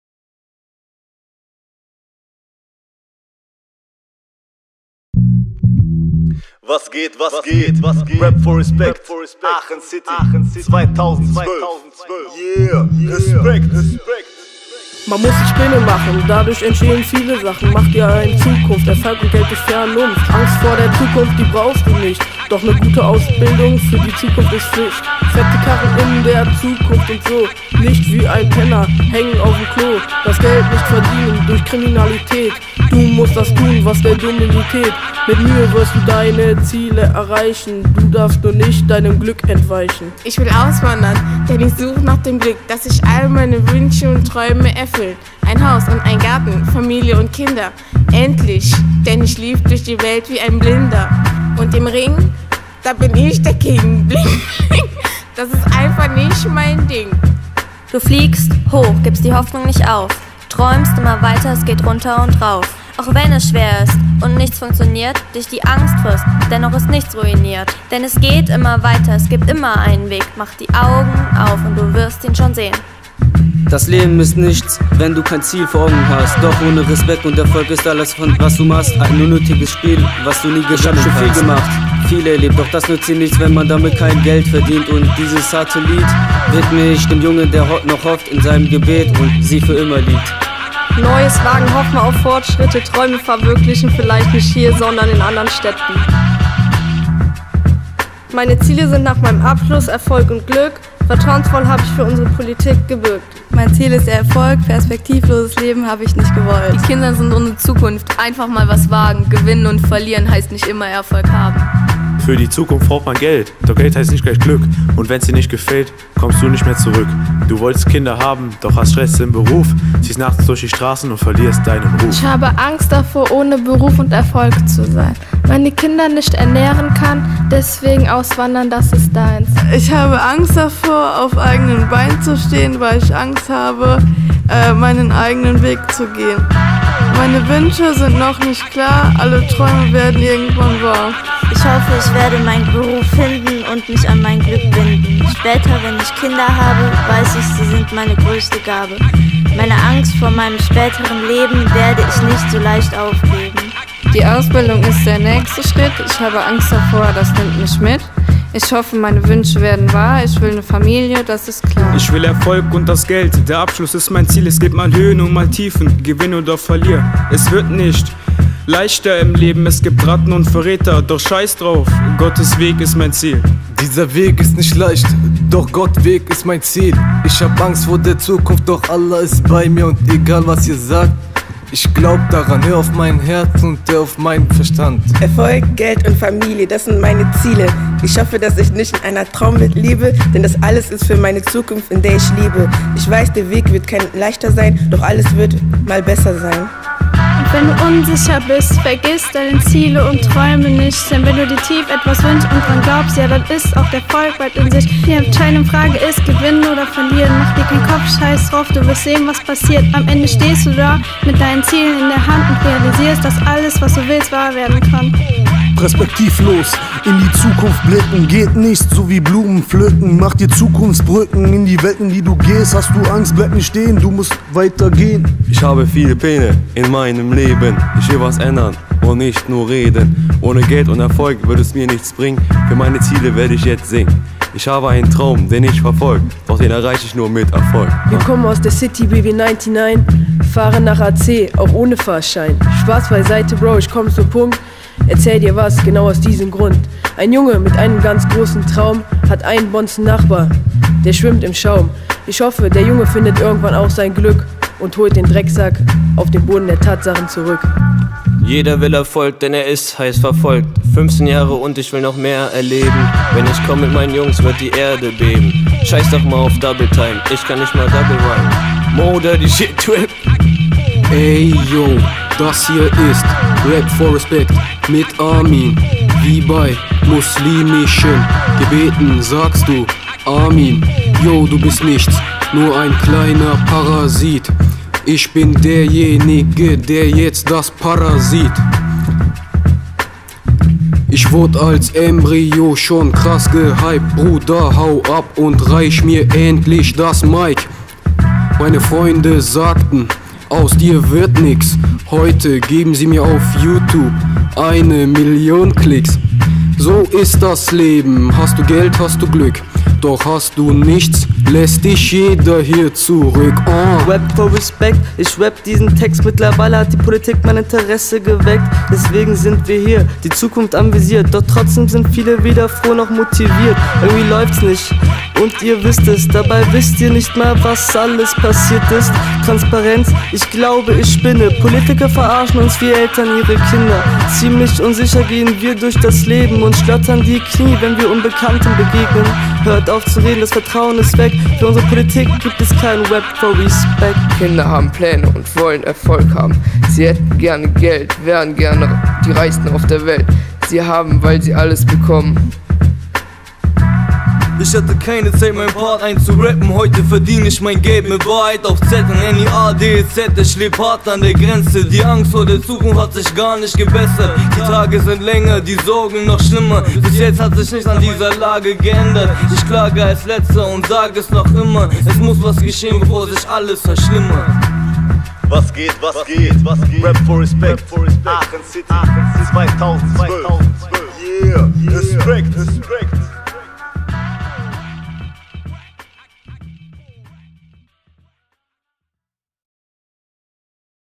Rap4Respect - Was geht - Aachen
Im Workshop in Aachen auf der Mitdenken, Mitreden, Mitmischen - "Was geht?! - Werkstatt für Jugendbeteiligung und Jugendpolitik" solten sich Jugendliche am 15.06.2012 im Eurogress in Aachen mit dem Thema Politik und Zukunft auseinandersetzen.
Es ging darum, dass Jugendliche ihre politischen Forderungen und Wünsche an diesem Tag zum Ausdruck bringen können. Einen Rap Song zu schreiben und aufzunehmen war für alle Jugenliche eine besondere und neue Erfahrung ihre Föderungen auf diese Art und Weise auszudrücken.
Insgesamt gab es an diesem Tag etwa 15 Workshops, zu denen sich die Jugendlichen um Vorfeld anmelden konnten. Zielgruppe der Veranstaltung waren Jugendliche aller Schulformen ab der 9.Klasse aus der StädteRegion Aachen.

Gefördert wurde der Rap4Respect Workshop von Jugend für Europa.

Kooperationspartner
"Was geht?! ist eine Kooperationsveranstaltung von:
Rat der deutschsprachigen Jugend (Deutschsprachige Gemeinschaft Belgiens)
Europäisches Jugendparlament in Deutschland e.V.
Arbeitskreis Jugendpartizipation
Stadt Eupen
StädteRegion Aachen

Dieses Projekt wurde mit Unterstützung der Europäischen Kommission finanziert.